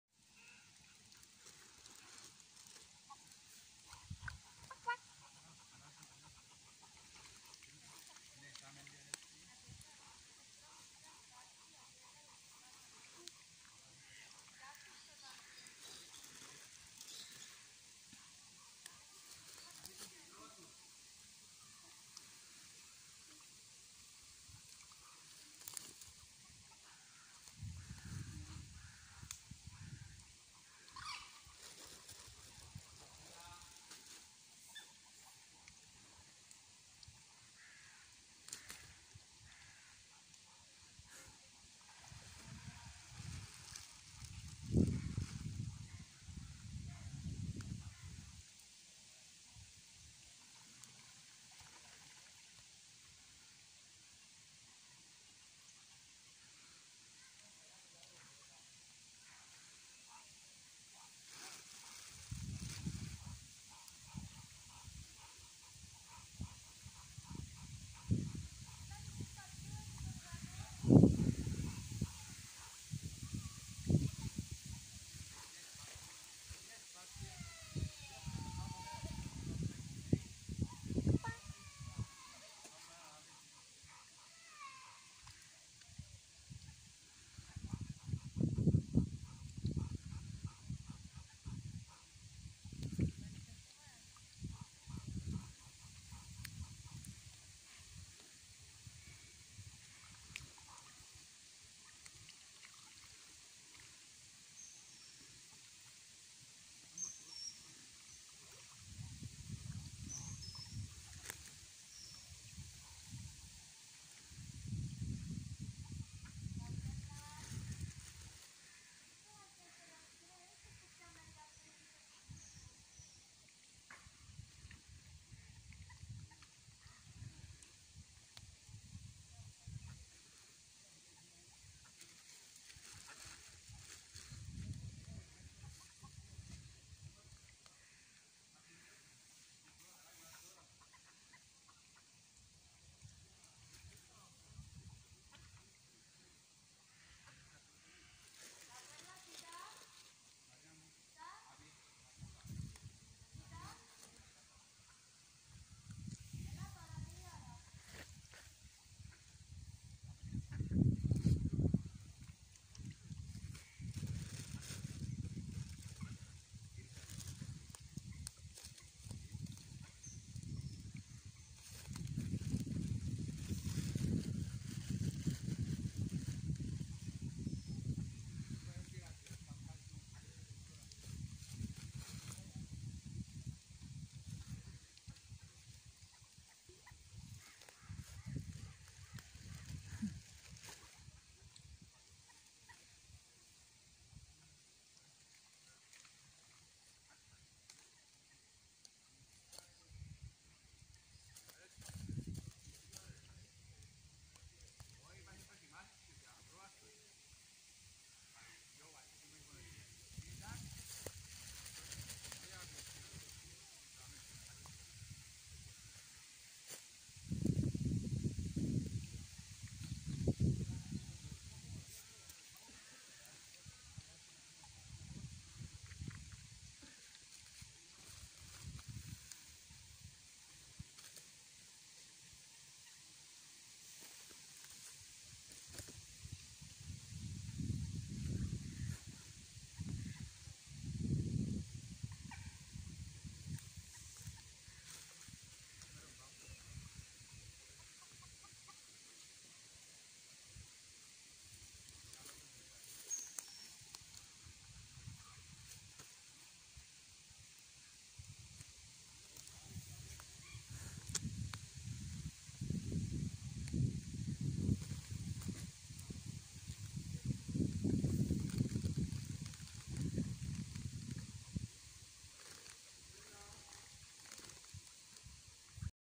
Majestic black duck.
Feeding ducks with bread. That black duck is  so beautiful and cute!